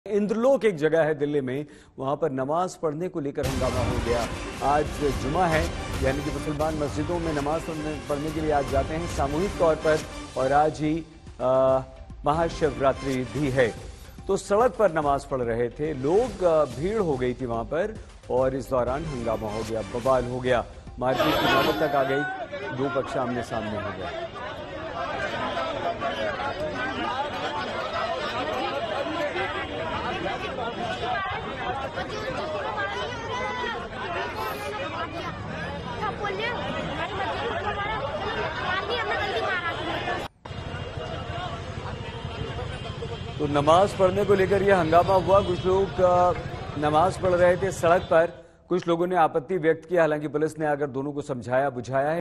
Breaking News: Delhi के Inderlok में सड़क पर नमाज़ पढ़ने के दौरान हंगामा | Delhi Police | Aaj Tak
Delhi के इंद्रलोक में सड़क पर नमाज़ पढ़ने के दौरान हंगामा. दिल्ली के इंद्रलोक में सड़क पर जुमे की नमाज चल रही थी.  मौके पर मौजूद कुछ लोग भड़क गए और हंगामा शुरू हो गया. कुछ लोग वीडियो बनाते रहे. मौके पर मौजूद लोगों ने पुलिसकर्मी को घेर लिया.